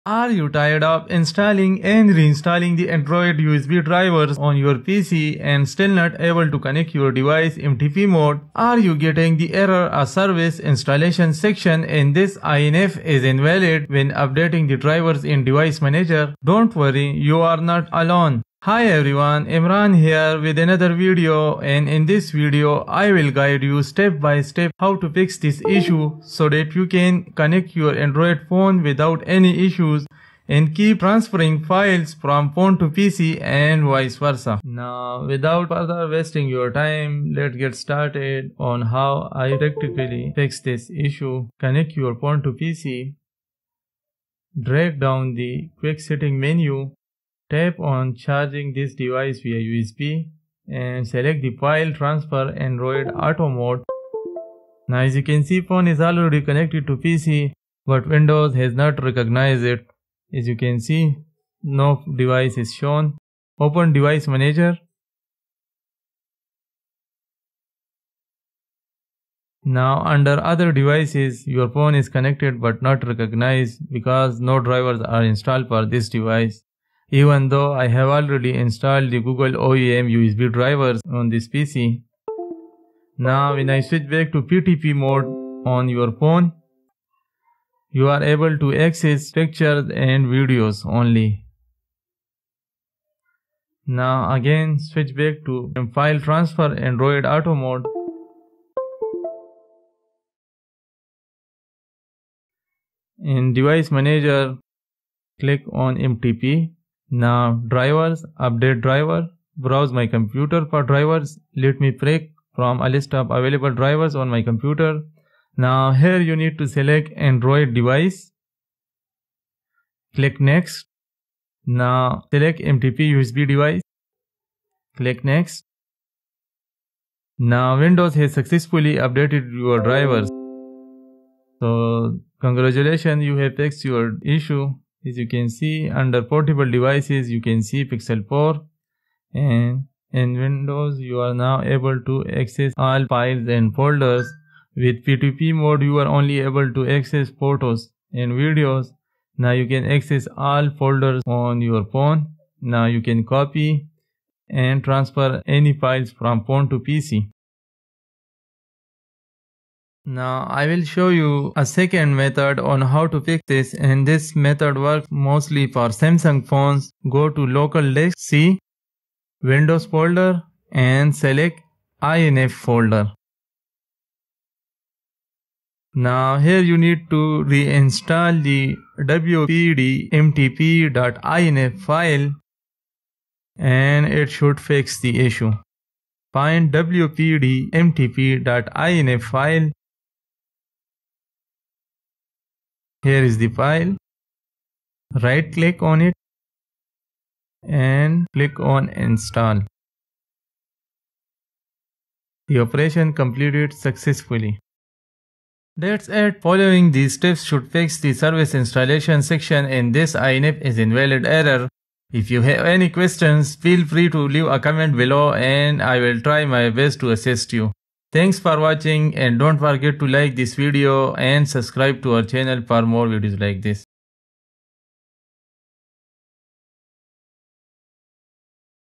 How to Fix a Service Installation section in this INF is invalid
A step-by-step tutorial on how to fix a 'Service Installation Section in this INF is Invalid' error. If you've encountered this error message while installing a new driver or software on Windows 11, Windows 10, Windows 8 and Windows 7 then don't worry, you're not alone.

In this video, we'll guide you through the steps to fix this issue and properly install MTP drivers on your Windows PC for all Android phones including Samsung, Google Pixel, One Plus, Oppo, Vivo, LG, Sony, Huawei, and Xiaomi smartphones.

After fixing the issue, transfer files between your PC without any issue.

More related tutorials you might like to watch:

How to Fix Google Pixel Error "Your device is corrupt. It can’t be trusted and may not work properly"